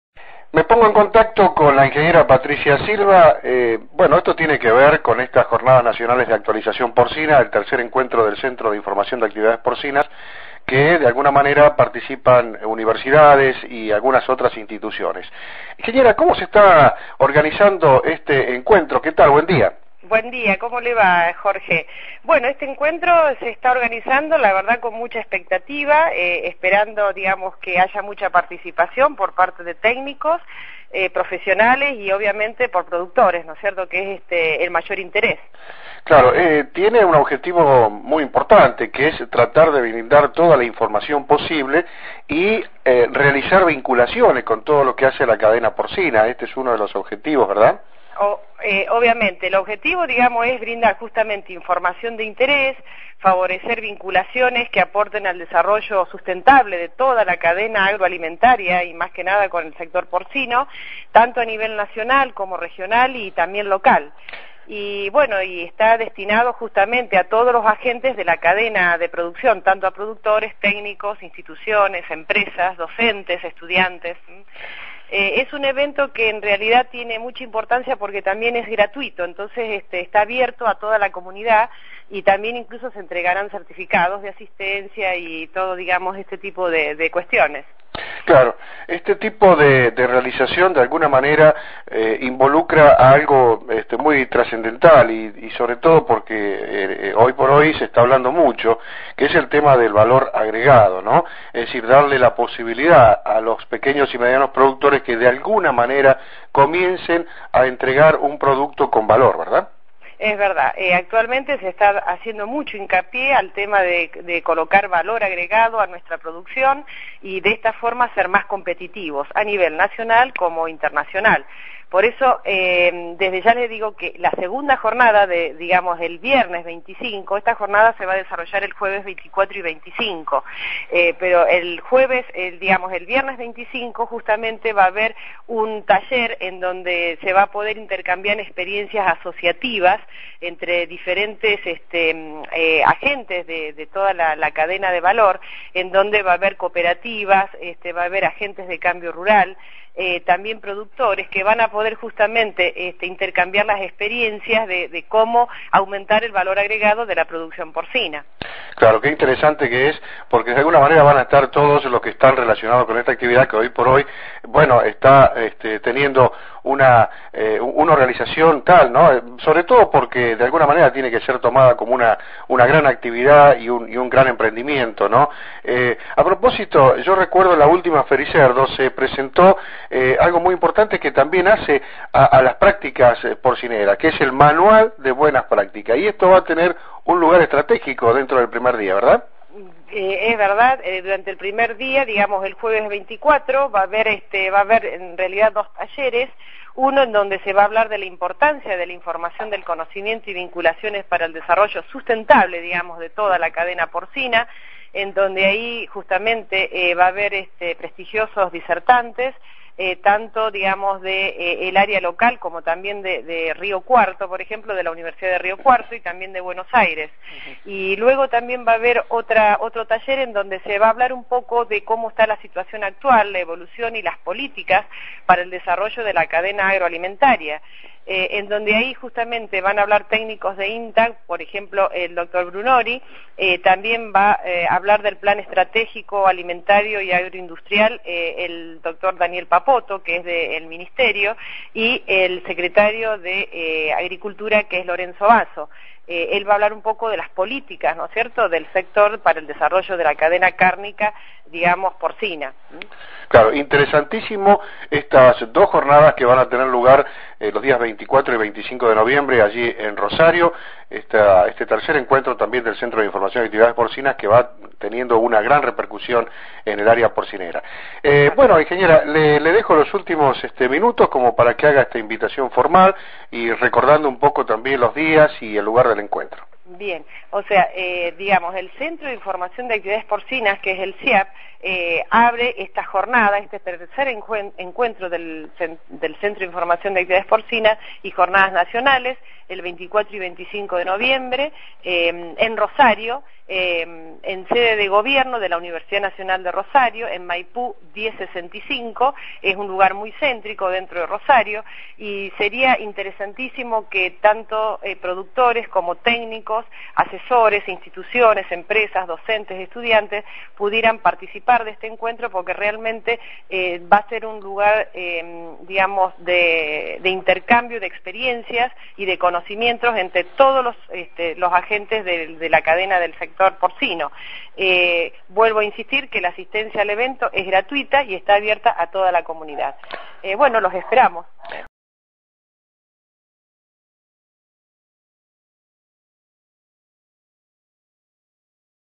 Jornada Nacional de Actualización Porcina - III Encuentro del CIAP (Rosario, 24-25/11/11)
La Ing. Patricia Silva (Universidad Nacional de Rosario) anticipa sobre este encuentro que se desarrollará durante los dias 24 y 25 de noviembre de 2011.